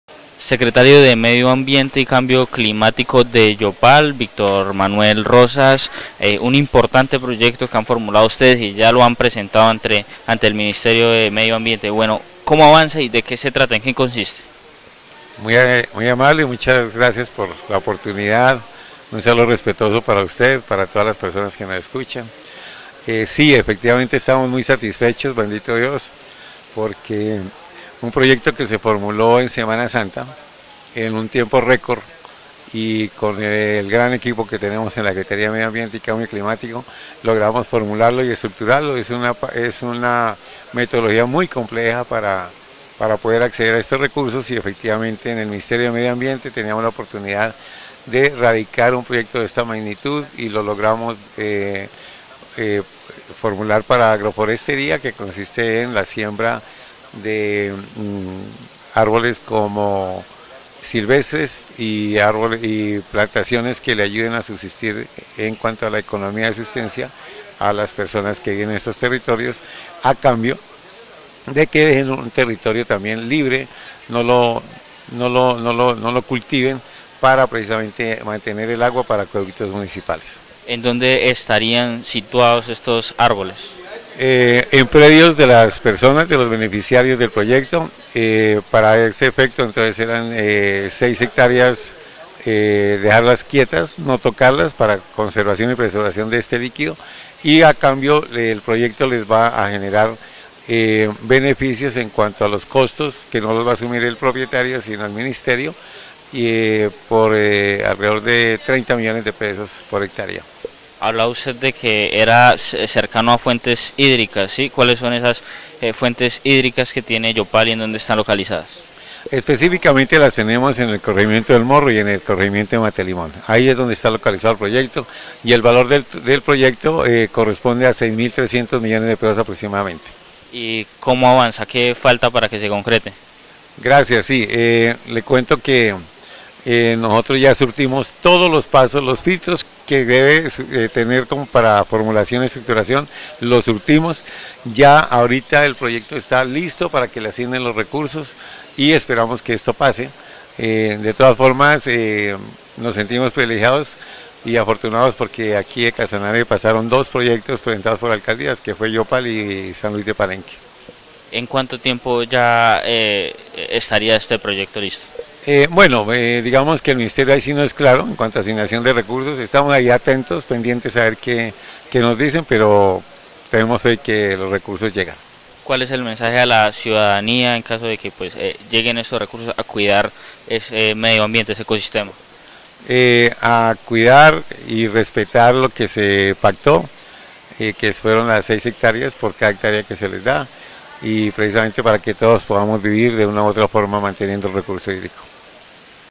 Victor Manuel Rosas - Proyecto de restauración ecológica ante MinAmbiente por más de $6300 millones
Alcaldía de Yopal logró viabilizar Proyecto de conservación y restauración ecológica ante el Ministerio de Medio Ambiente por más de $6300 millones que se ejecutarán en El Morro y Mata de Limón, según confirmó Victor Manuel Rosas, Secretario de Medio Ambiente y Cambio Climático